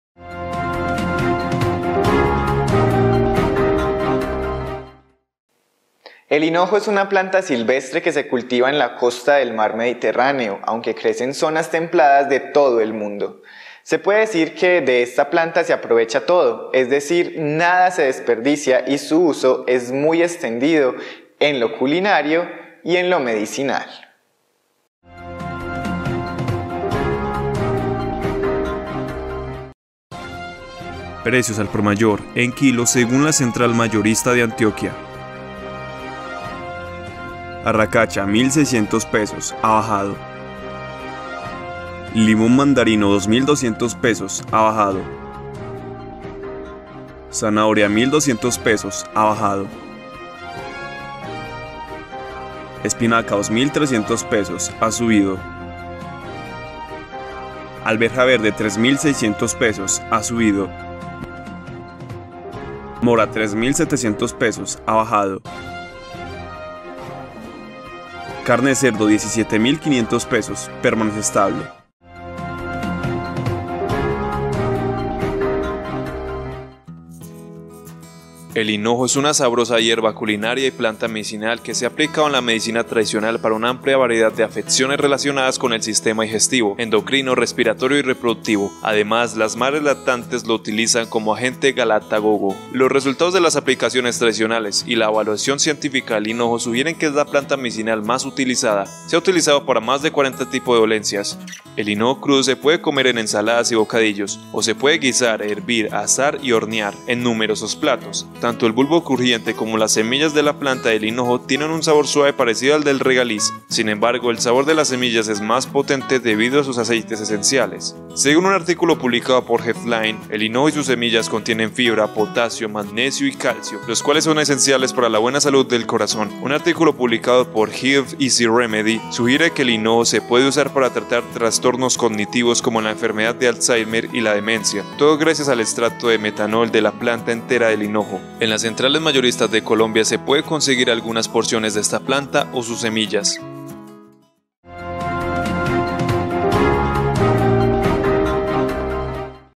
TIP SEMANAL: EL HINOJO
El hinojo es una planta silvestre que se cultiva en la costa del mar mediterráneo, aunque crece en zonas templadas de todo el mundo. Se puede decir que, de esta planta se aprovecha todo, es decir nada se desecha y su uso es muy extendido en lo culinario y en lo medicinal.